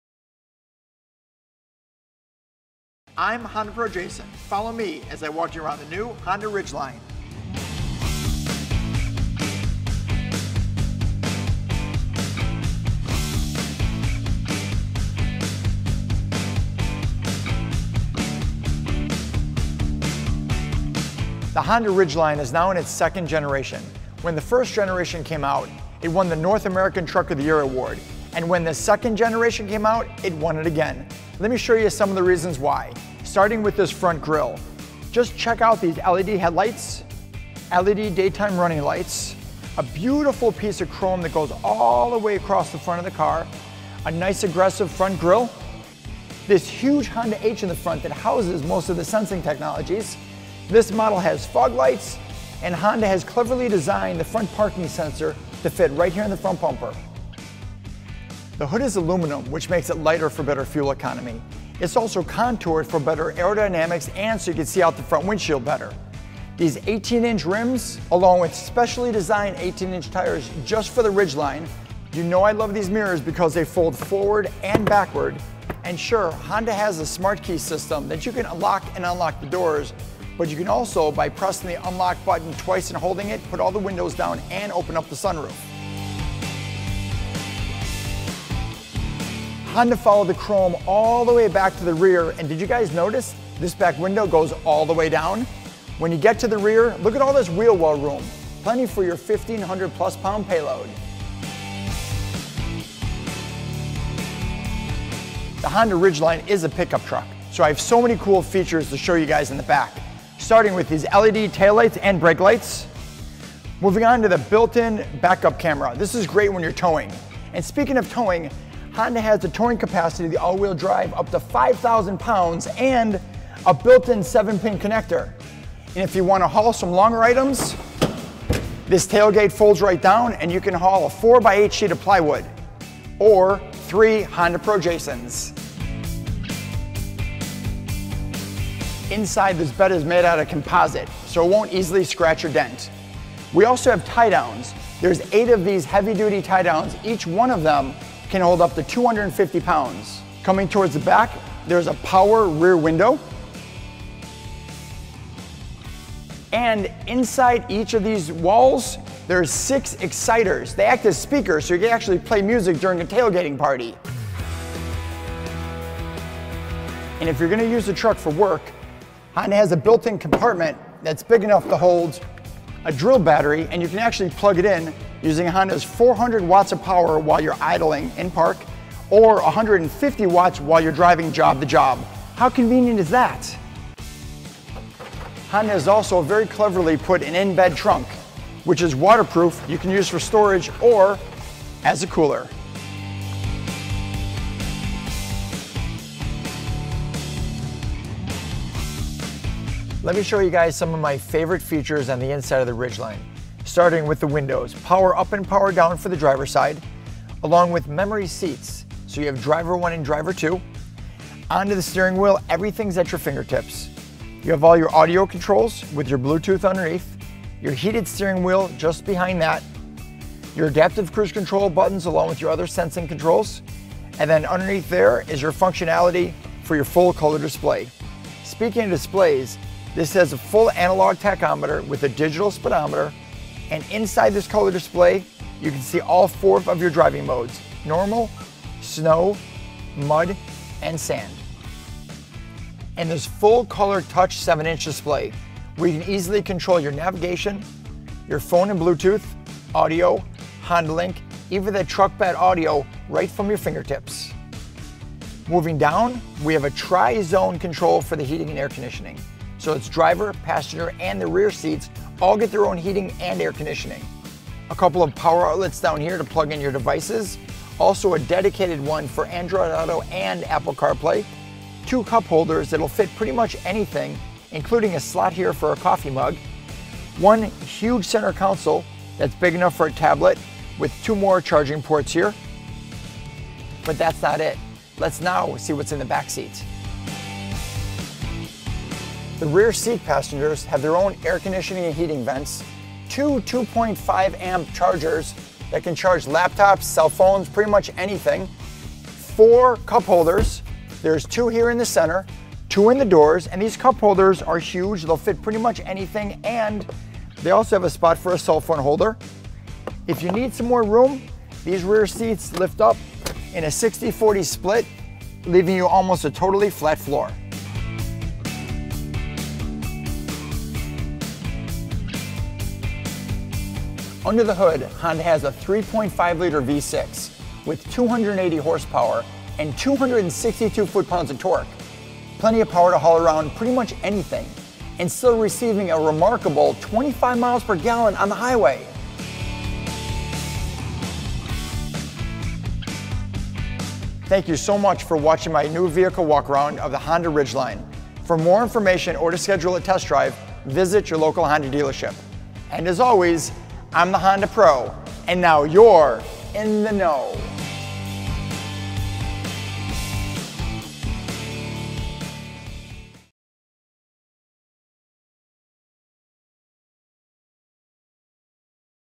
HondaPro Jason - 2018 Honda Ridgeline Walk-Around
The 2018 Honda Ridgeline is the most versatile truck on the road. With a built in cooler and speakers in the bed, the Ridgeline is the perfect truck for a tailgate. Need a truck that can tow your boat? The Ridgeline has a towing capacity of 5,000 lbs. Find yours at Honda North today!